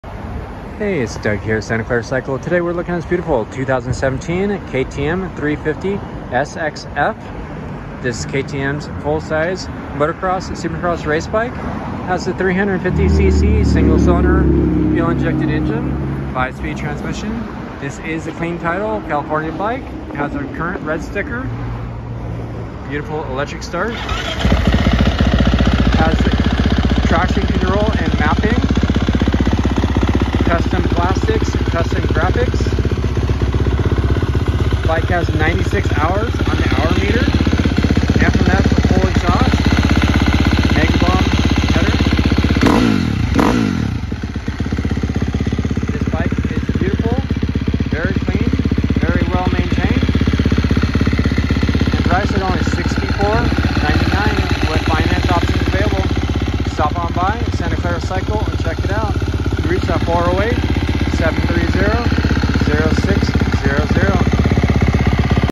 2017 KTM 350 SXF ... Very Clean " Ready to Race " in Northern CA
2017 KTM 350 SX-F
*clean CA title , Red sticker
*350cc single cylinder fuel injected engine, 5 speed transmission , electric start
*96.5 hours
*lots of upgrades: FMF full exhaust Megabomb header w 4.1 pipe, custom plastics ,graphics and seat cover , Pro Taper bars and lock-on grips, Santa Cruz Suspension set for 160 lb rider and more
*Vet owned and well maintained
*ready to race, ride or look great in the living room!
*Similar to the Honda CRF450r , KTM 350 XCF and Husqvarna FC 350 models
This killer KTM is offered at $6499  w financing options available w rates starting @ 0% apr for 15 months!

You can see this used motorcycle and many more @ Santa Clara Cycle in Sunnyvale!